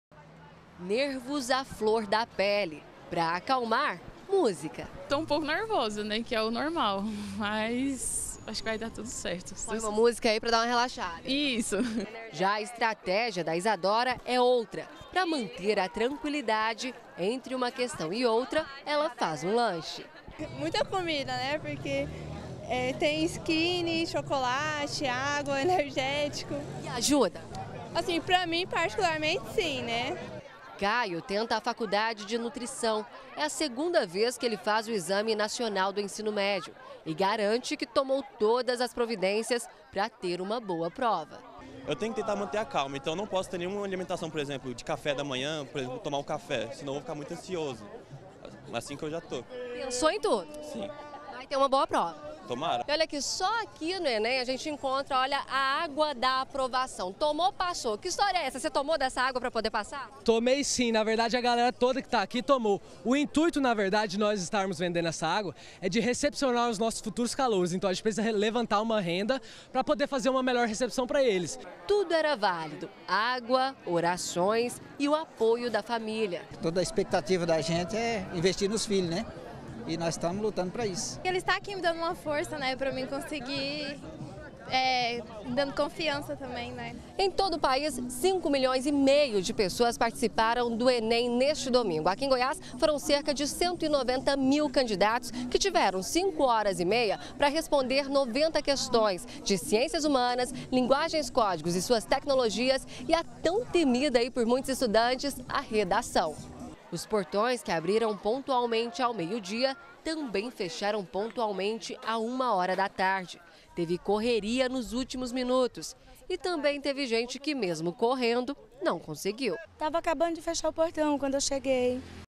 ENEM: 190 mil candidatos fizeram provas em Goiás
Cento e noventa mil candidatos do ENEM em Goiás fizeram prova ontem. Eles tiveram cinco horas e meia para responder noventa questões. Na porta dos locais de prova, teve candidato nervoso, outros acompanhados de familiares e, também, quem perdeu a hora e não conseguiu entrar para fazer o exame.